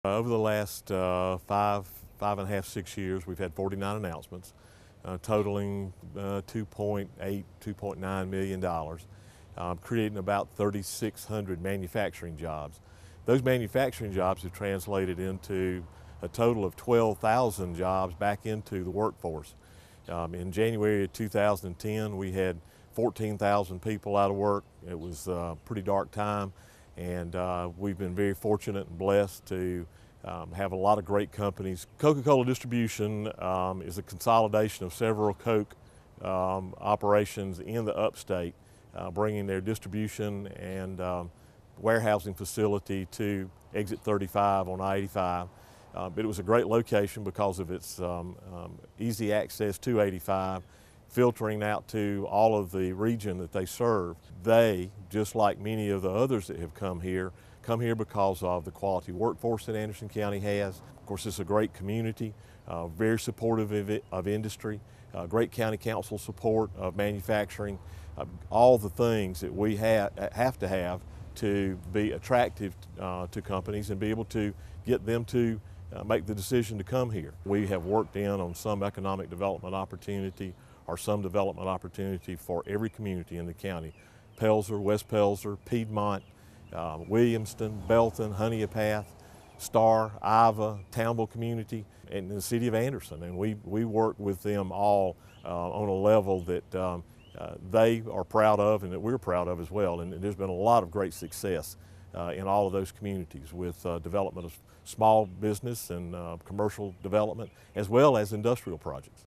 Grand Opening of Coca-Cola Sales & Distribution Facility Highlights Economic Boom
The Grand Opening of the Coca Cola Bottling Company Consolidated Sales and Distribution Facility in Anderson County highlights the continued trend of economic growth and development that has resulted in the recently announced unemployment rate of 4.4%.
Economic Development Director, Burriss Nelson, discusses the thriving environment that makes Anderson County such a great place for businesses to locate operations.
Over the last 5 years, Anderson County has experienced 49 major announcements representing more than $2.9 Billion in investment and the creation of nearly 4,000 new jobs. Today's Coca Cola opening marked the completion of a project that resulted in $13.5 Million invested in the County and an increase of more than 225 jobs. We want to apologize to Burriss for leaving off the other 's' in his name! Sorry!

Periscope: ACTV193